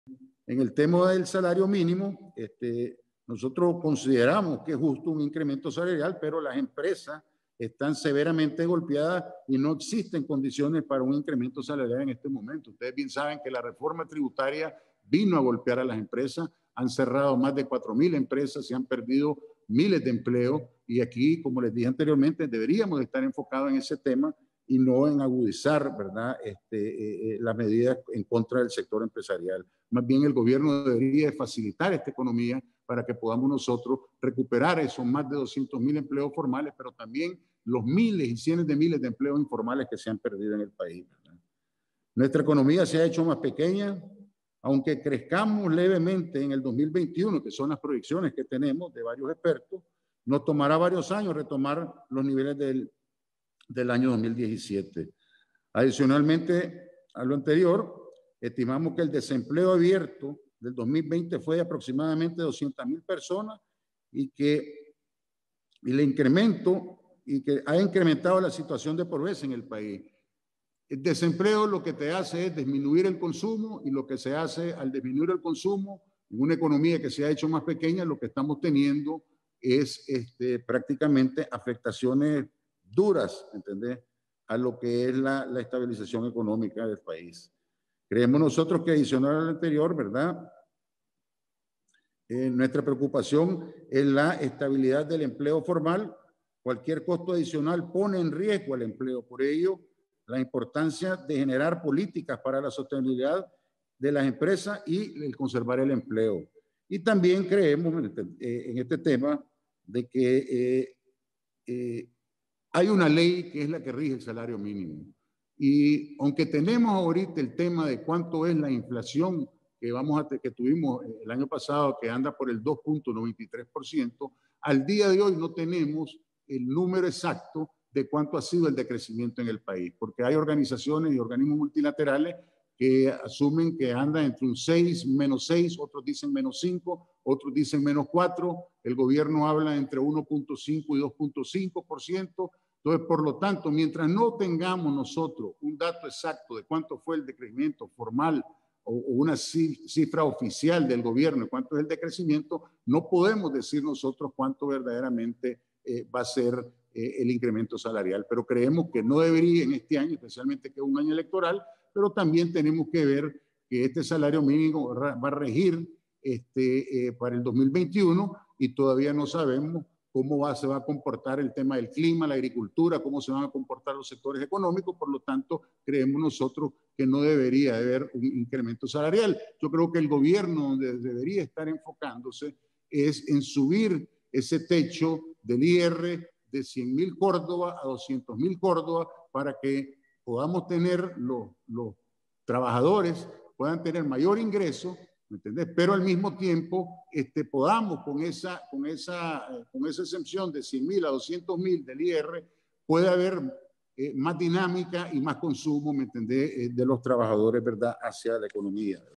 "Aunque debería haber un incremento en el ajuste salarial no existen condiciones para hacerlo"
Michael Healy presidente de la cúpula empresarial dijo que, aunque debería haber un incremento en el ajuste salarial no existen condiciones para hacerlo.